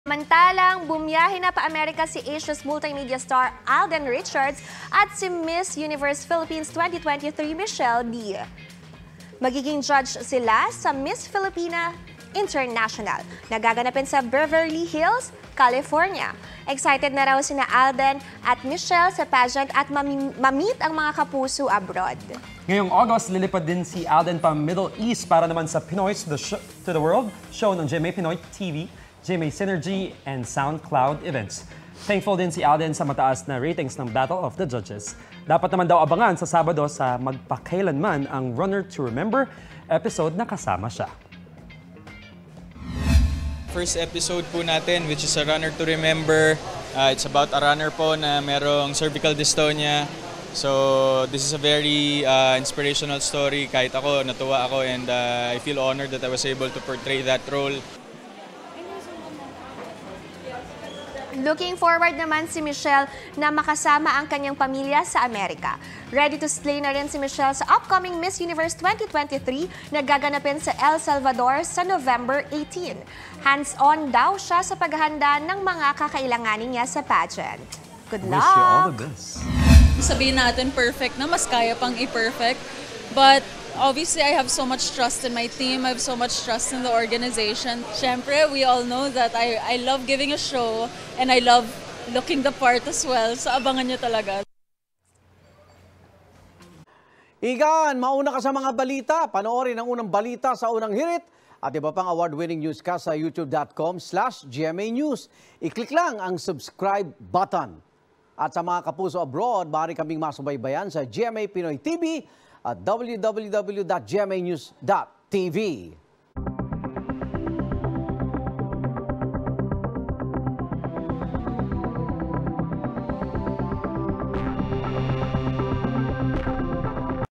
Alden Richards at Michelle Dee, magiging judge sa Miss Filipinas International... | UB
Alden Richards at Michelle Dee, magiging judge sa Miss Filipinas International na gaganapin sa Amerika